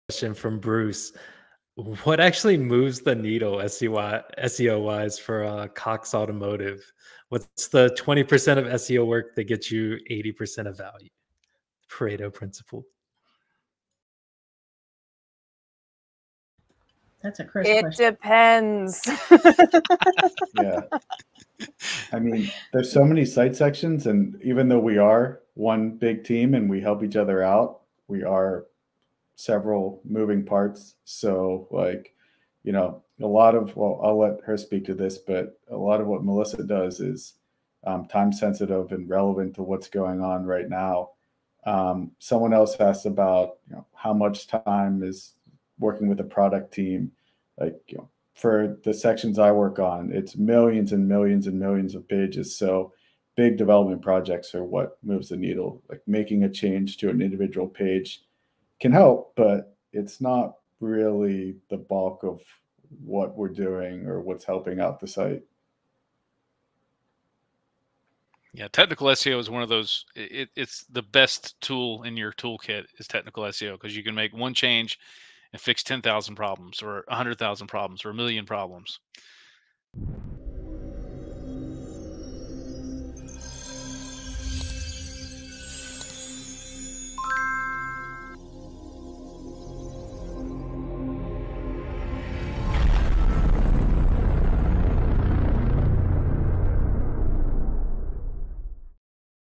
What actually moves the needle of SEO at Cox Automotive
Bruce Paulson (Owner of Determined Solutions SEO) asked, "What moves the needle SEO wise for Cox auto? What is the 20 percent of SEO work, that gets you 80% of value?"

Ellen provided the typical answer, which also applies here, "it depends."

Matthew added that these projects have several moving parts. Most of what Melissa works on is time sensitive and relevant to current events. He expanded that working with the product team involves big development projects that impact millions of pages.

Keith expanded that technical SEO is one of the most impactful tools because you can make one change and it can impact 10,000 or one million pages.